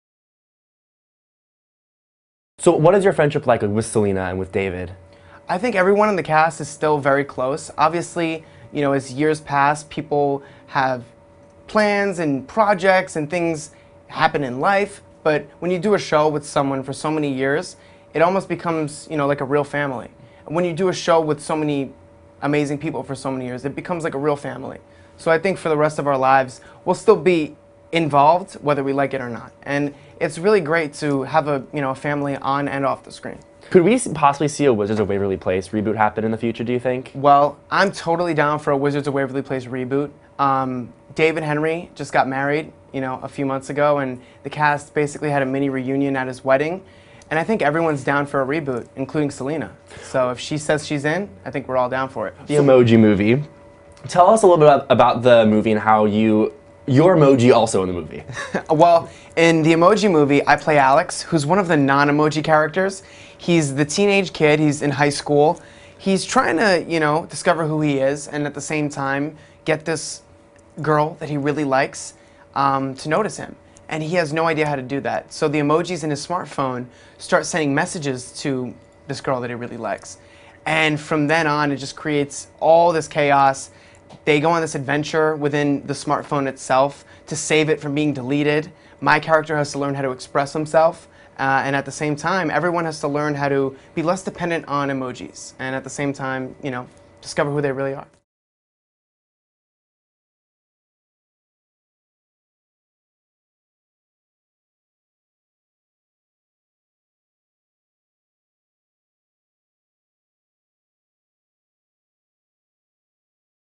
Jake T. Austin On His Lasting Friendship With Selena Gomez & David Henrie
"The Emoji Movie" star and "Wizards of Waverly Place" alum Jake T. Austin sits with AccessHollywood.com and talks about his friendship with "Wizards" co-stars Selena Gomez and David Henrie. Plus, would he be interested in a "Wizards" reboot? And, he talks about "The Emoji Movie."

Jake T. Austin On His Lasting Friendship With Selena Gomez & David Henrie | Access Hollywood